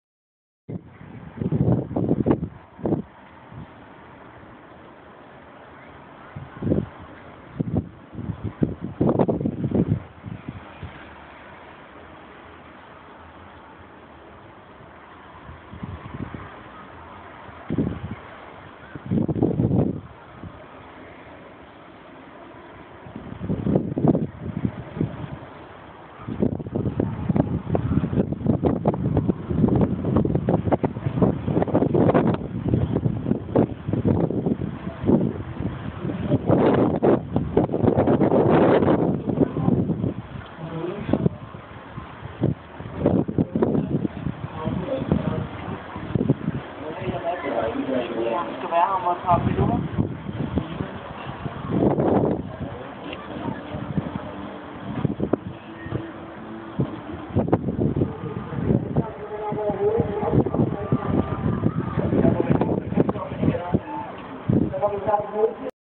Udsigt Bellahøj
Udsigt taget fra vores svalegang på Bellahøj Højhuse